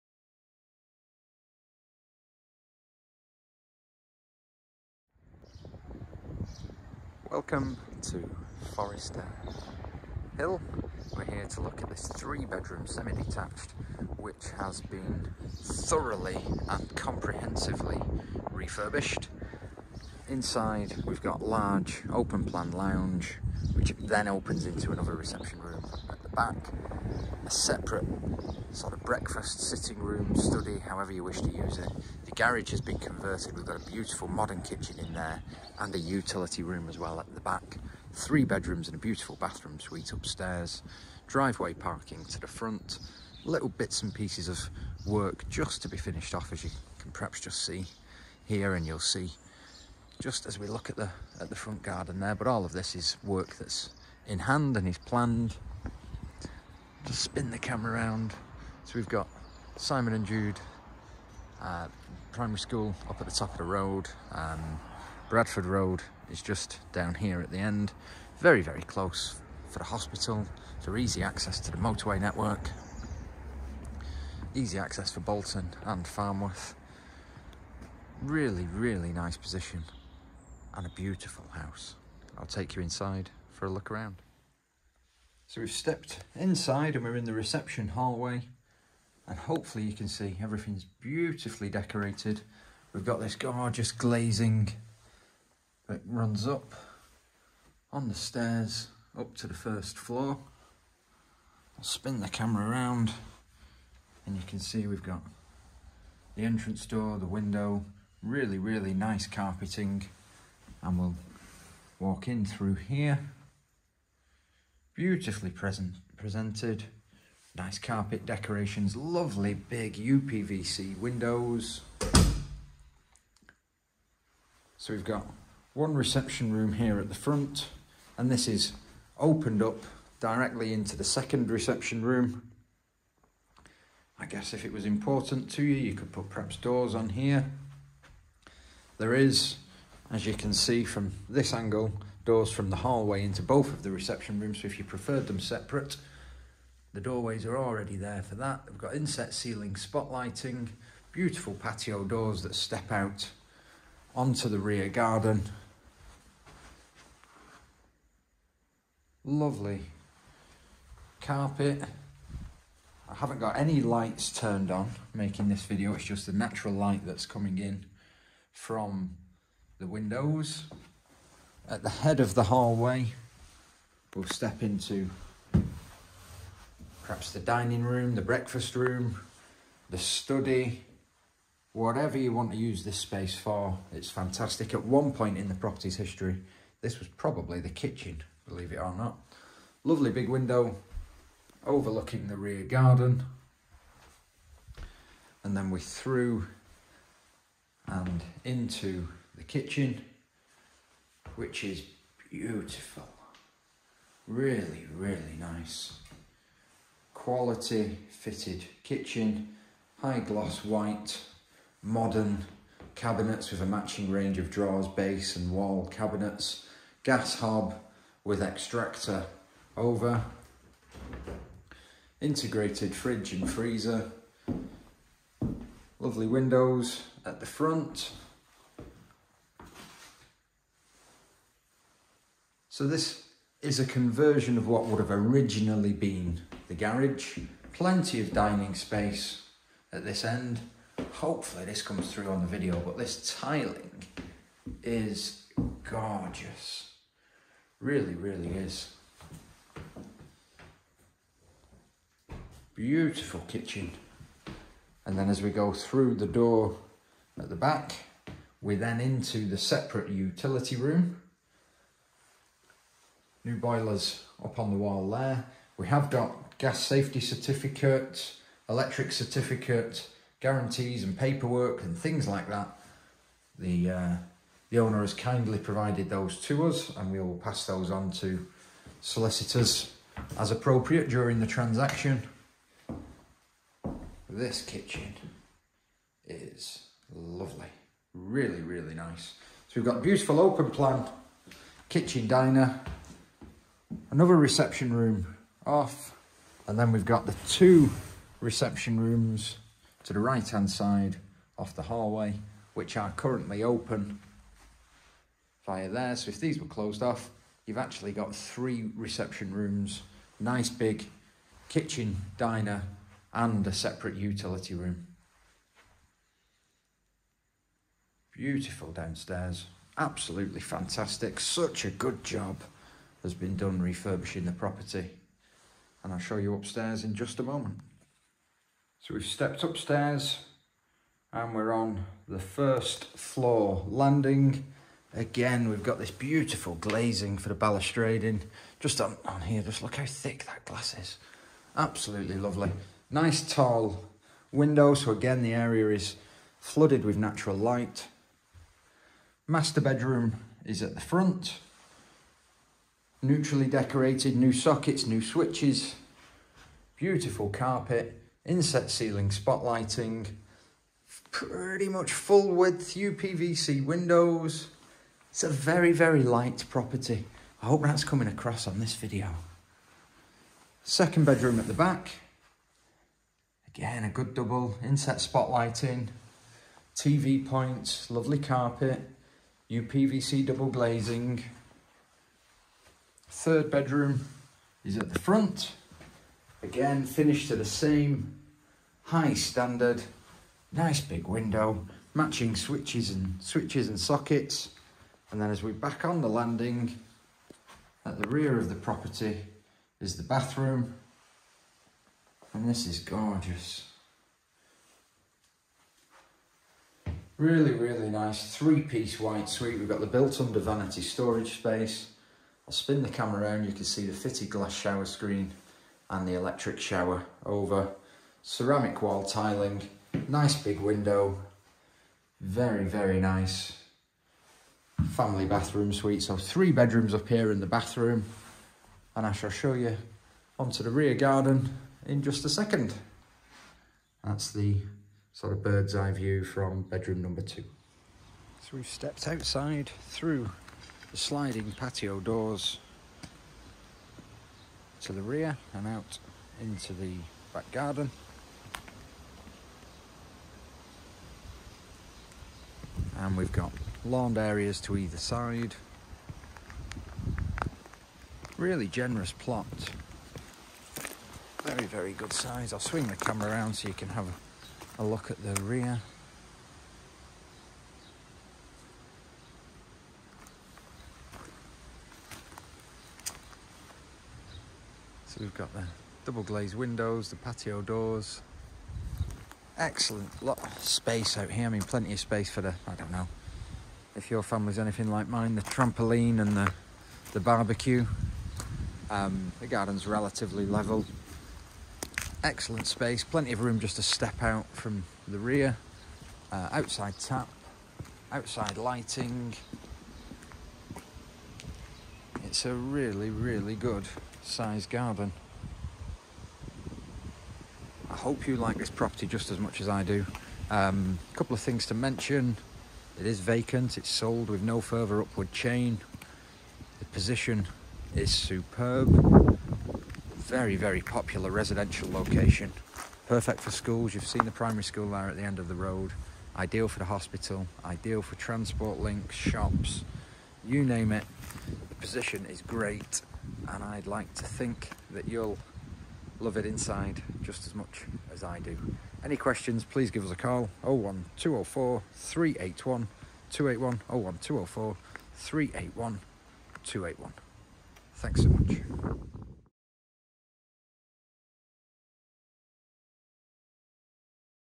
Forrester Hill Avenue, Bolton
A recently comprehensively refurbished three bedroom semi-detached property which is in superb condition throughout hopefully ready to move straight into. Recent works completed including converting the garage to further living accommodation, wiring, new boiler, decorations, plastering, new kitchen, new bathroom etc.

The family home is set in a consistently popular residential location within a short walk of one of the areas most popular primary schools, close to Bolton Hospital, and within easy reach of beautiful local countryside, shops, transport links, houses of worship, sporting facilities and recreational facilities.

The accommodation is particularly well presented throughout and briefly comprises: reception hallway, living room, which opens into the sitting room, morning room, beautiful modern white kitchen/diner, utility room, first floor landing, three bedrooms and a beautiful white three-piece bathroom suite. There is gated off-road driveway parking to the front and a generous size enclosed rear garden.

Importantly the family home is offered for sale with early vacant possession and no further upward chain delay, therefore it is hoped a prompt completion can be arranged once a sale is agreed. Viewing is essential to appreciate all that is on offer and the potential. In the first instance a walk-through viewing video is available to watch and then a personal viewing can be arranged by calling Cardwells Estate Agents Bolton on (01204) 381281, emailing: or visiting